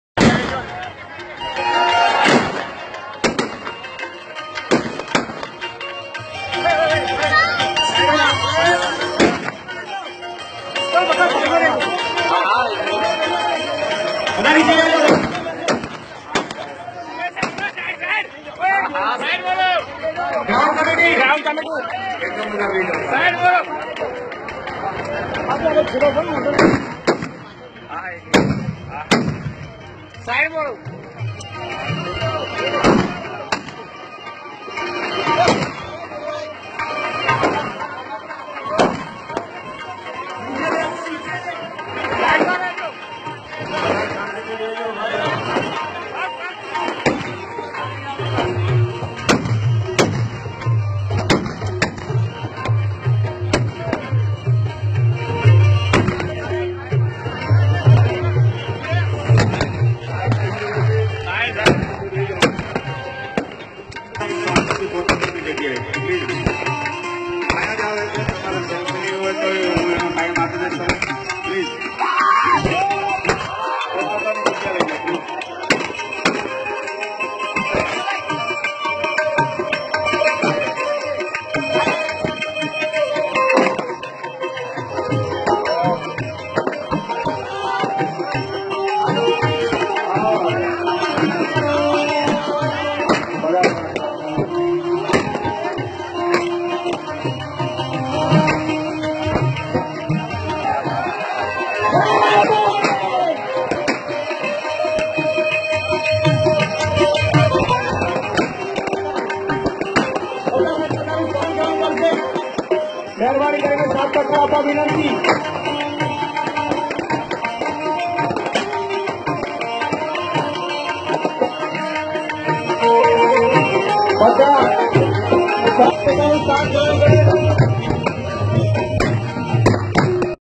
ગિર ના સાવજ રાજભા ગઢવી નુ આવુ સ્વાગત રાજપૂતો યે કરી બધૂક ની ગોળી વરસાવી ને
‎Tusky તરફથી વિડિયો